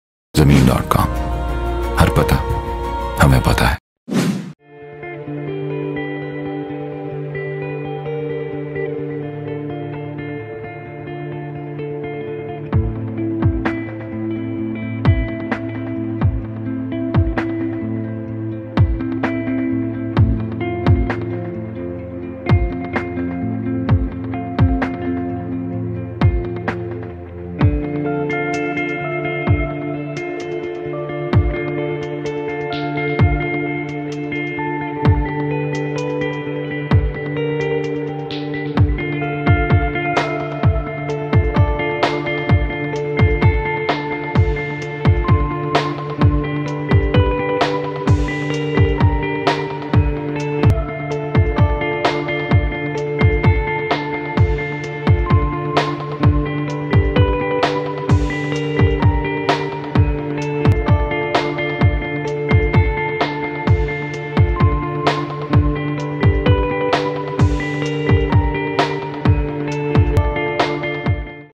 5 MARLA RESIDENTIAL PLOT FOR SALE AT HASILPUR ROAD BAHAWALPUR
Such a nice located 5 Marla Residential Plot is available for sale on Hasilpur Road, Bahawalpur.

Gold Glass Cinema & Bank.
Cash and carry super market, central park.
3.13 Acre.
Ladies and gents Gym.
8 Kanal Jama Masjid, Swimming Pool, Hospital, School, Security Office, Open Gym, Mini Zoo, Library, Food court & Restaurant.
Main Gate, CCTV Cameras, Basekit Ball Courts, Football Courts, Electricity, Gas, Water Filtration Plant.